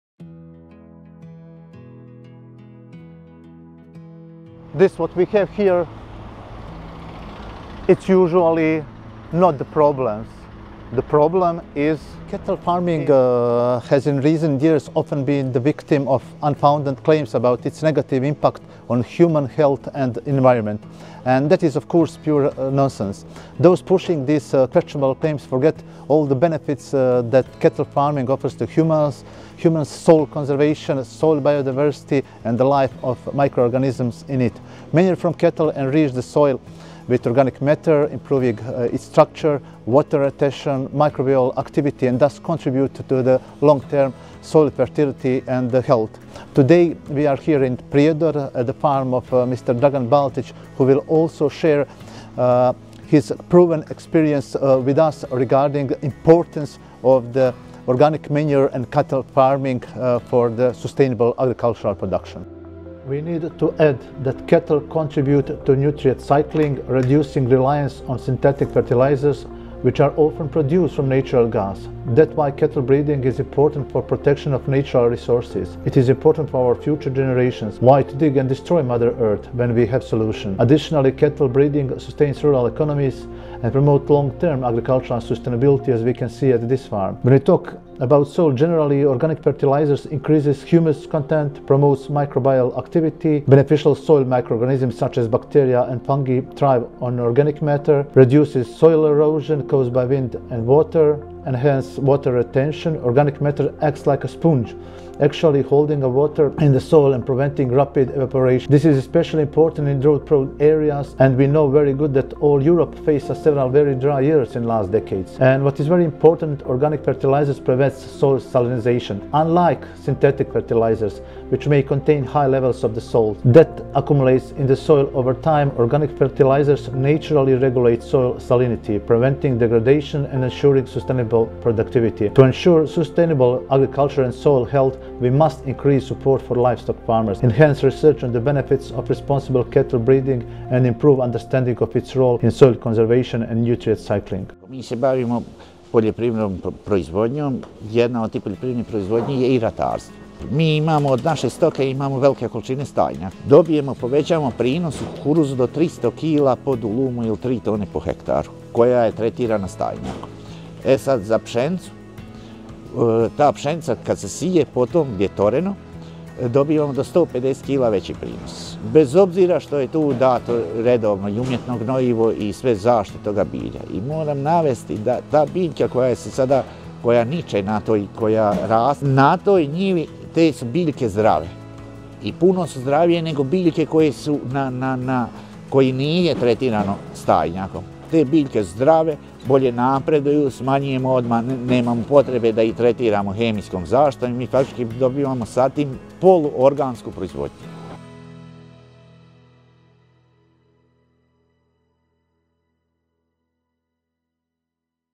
Sustainable management of agricultural soil
This video was created within the PLUS Change project, funded by the Horizon Europe programme, to support the development of sustainable land use strategies.
This video is prepared by the Dairy Farmers Association of the Republic of Srpska.The goal of the video is to highlight the importance of manure for preserving the soil and the life in it.